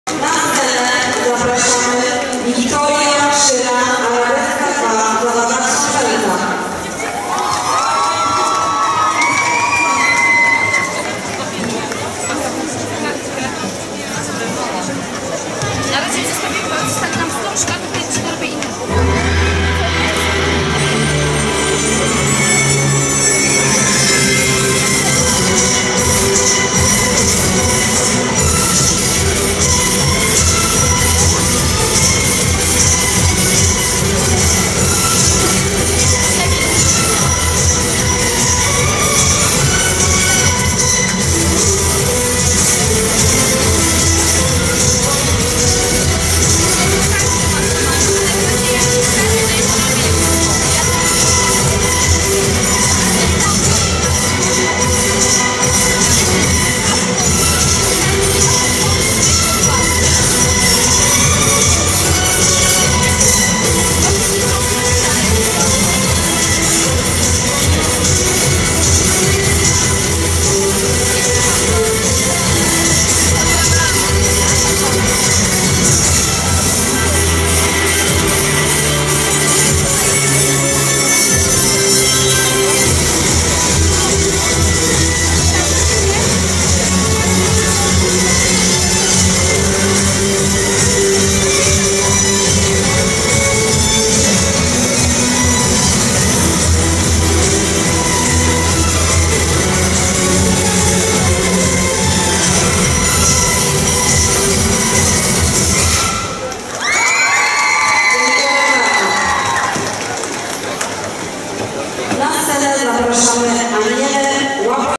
Wiktoria Szyra - Arabeska II MP 2014 solo 2xbaton
Wiktoria Szyra - mażoretki Arabeska II (Zawada Książęca, Polska), 3 miejsce w kategorii solo 2xbaton kadetki, XVI Mistrzostwa Polski Mażoretek, Kędzierzyn-Koźle, 24 maja 2014 r.